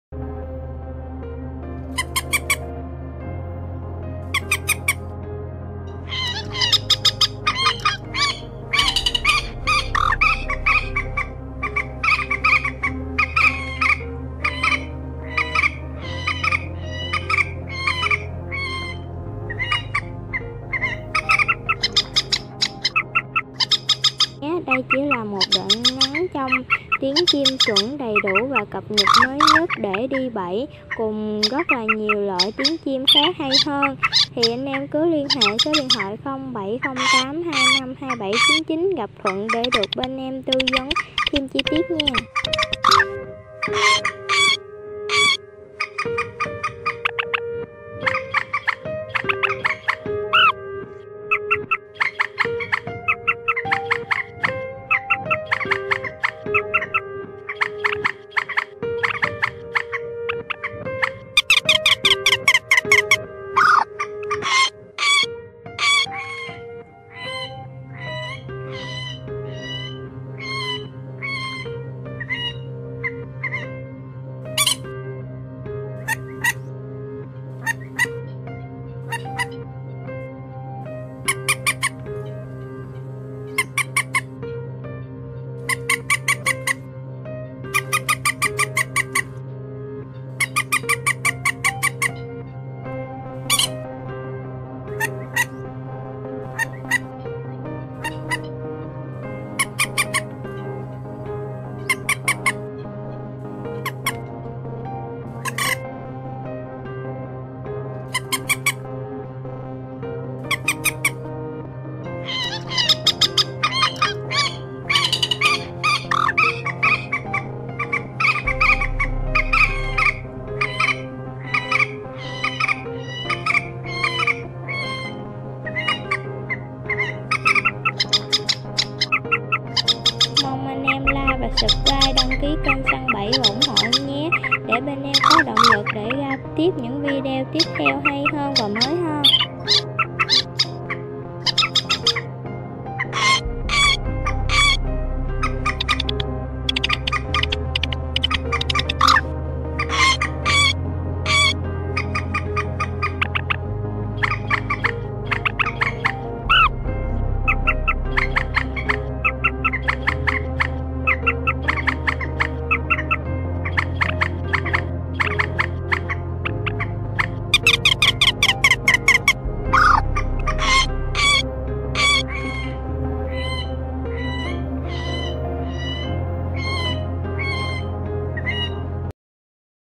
Tiếng Trích Ré , Quốc Biển , Chim Quốc Đen , Quốc Móc Tiếng Chuẩn Tháng 8 /2019
Đã vào mùa chim tháng 8 /2019
rồi ae đã chuẩn bị tiếng trích và tu hú và nhiều loại khác chưa .